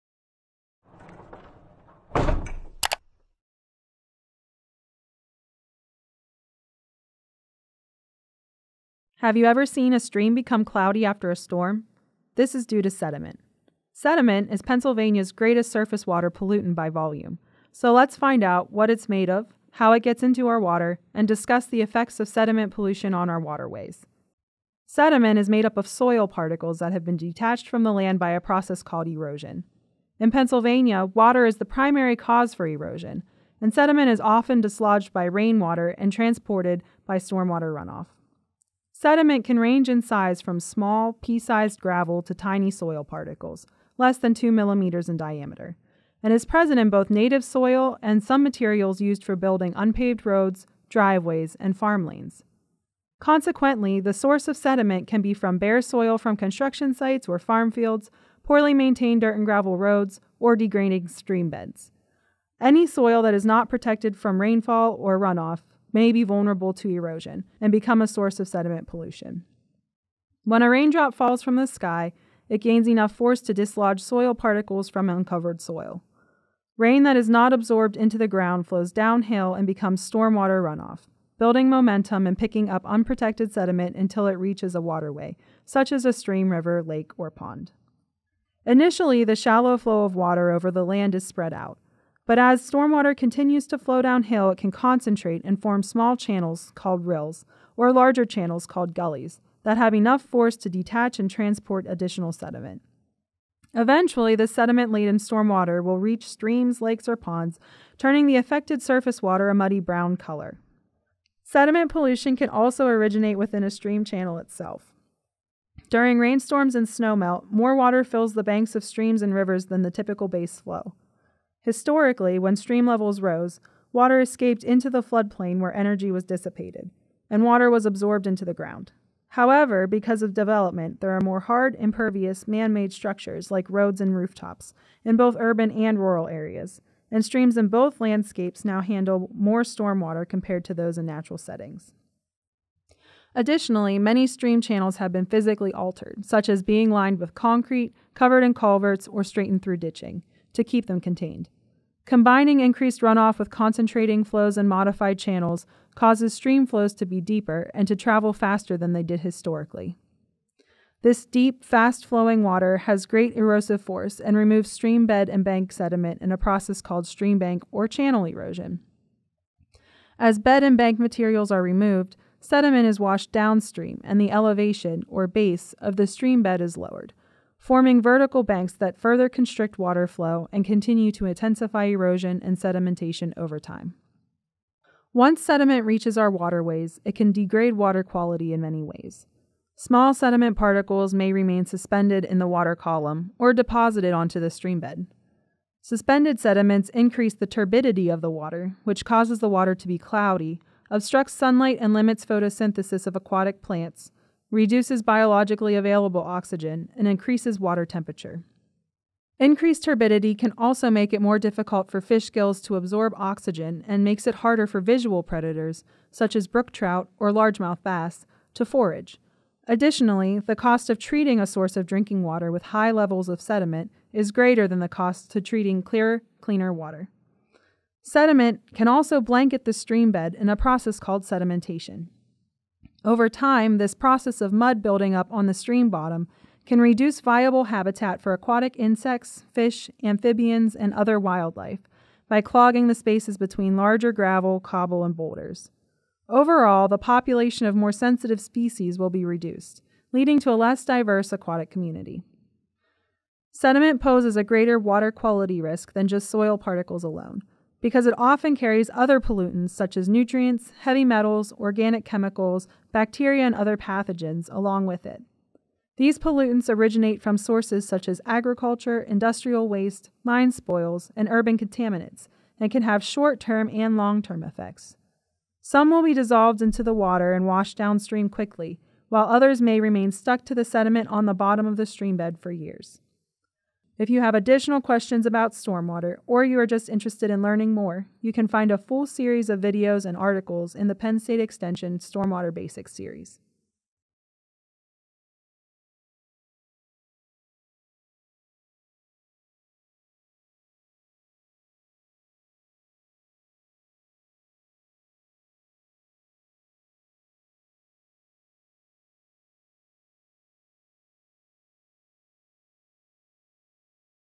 What is Sediment and Why is it a Stormwater Pollutant?
Sediment is a stormwater pollutant that is made up of soil particles that have been detached from the land by erosion and is Pennsylvania’s largest surface water pollutant by volume.